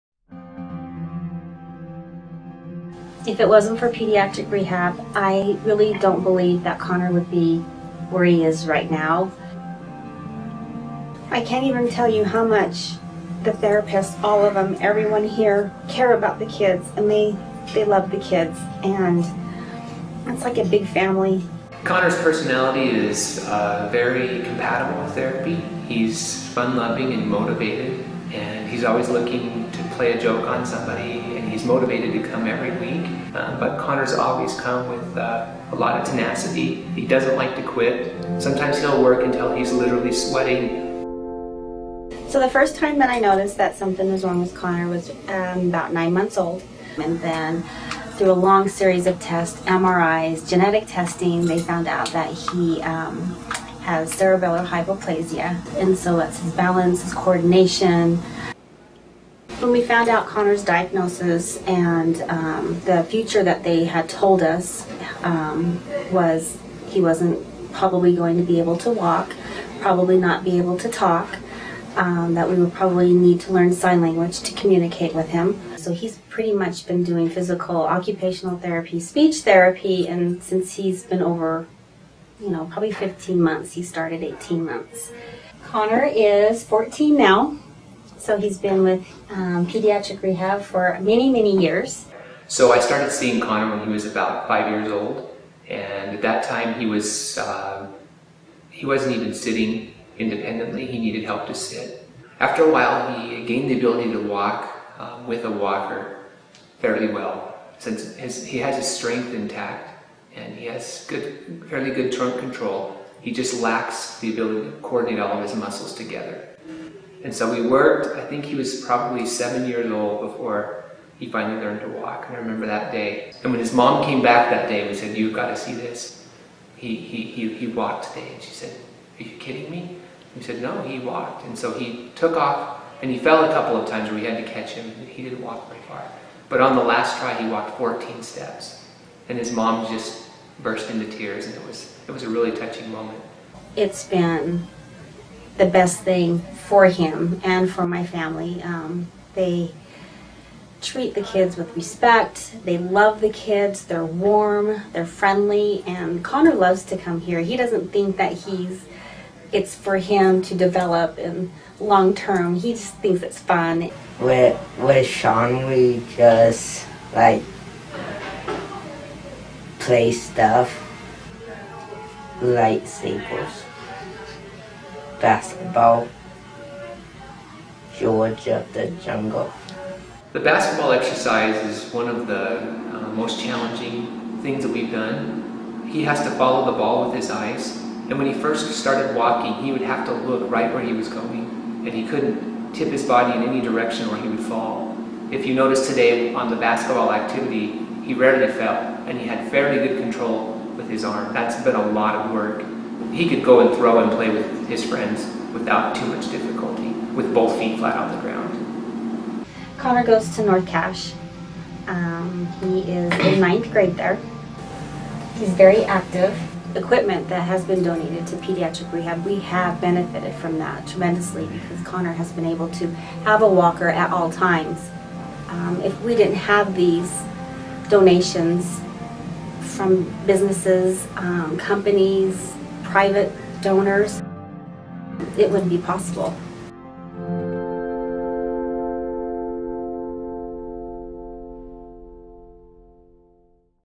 Logan Regional Pediatric Rehab
Logan Regional Hospital's Pediatric Rehab works to help kids advance their motor skills and expand their movements. The Logan Regional Hospital Foundation has helped fund equipment for Logan Regional Hospital's pediatric rehab unit.